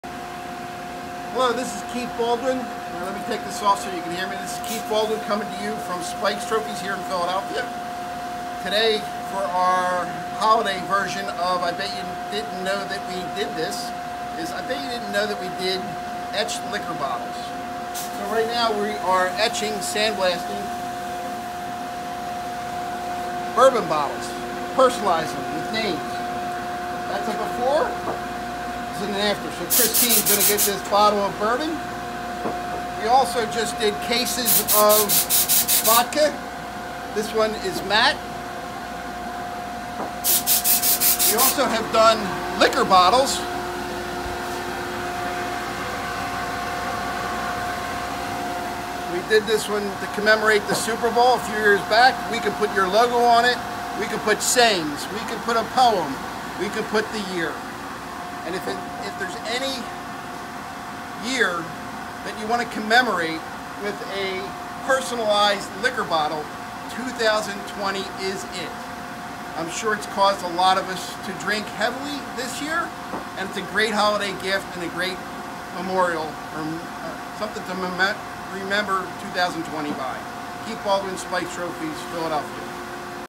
"I Didn't Know You Did Etched Liquor Bottles!"
Did you know we etch liquor and wine bottles? We do! Bring us the bottle and we'll bring the personalization to make it memorable! We'll do one bottle or  cases, and can personalize them with a name or a logo. The personalized bottle will remain long after its contents are gone.

Spike's Trophies can create a unique & memorable gift for an unforgettable year — a year that has caused many of us to have a drink or two! Find out more about some of our offerings